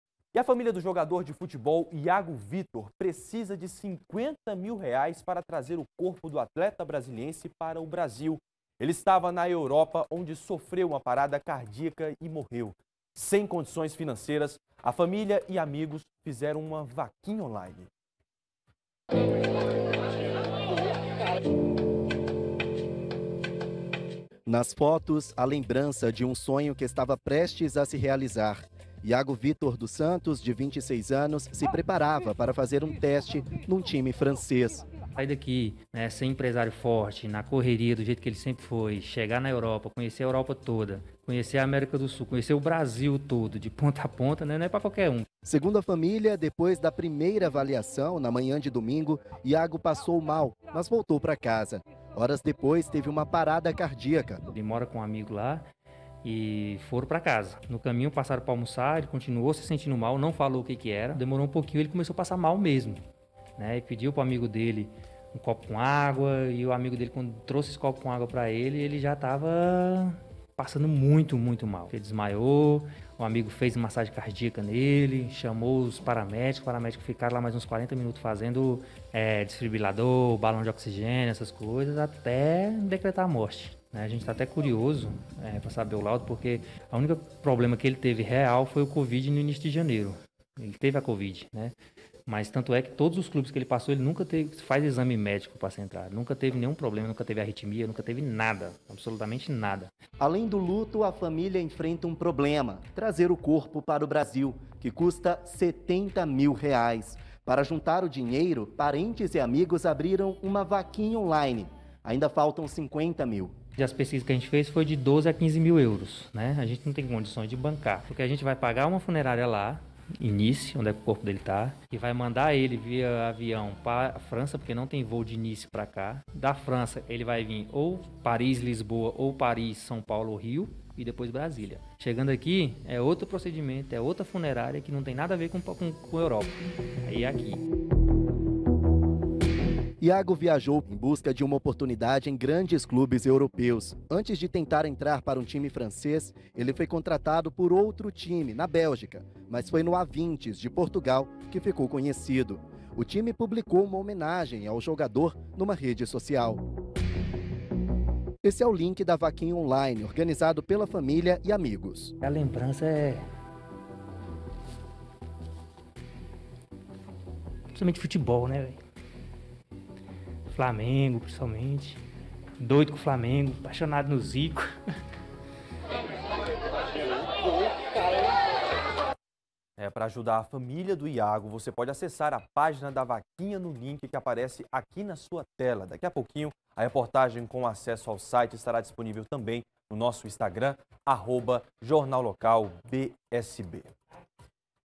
JL - Parentes pedem ajuda
02/03 - Família precisa de R$ 50 mil para trazer corpo do jogador.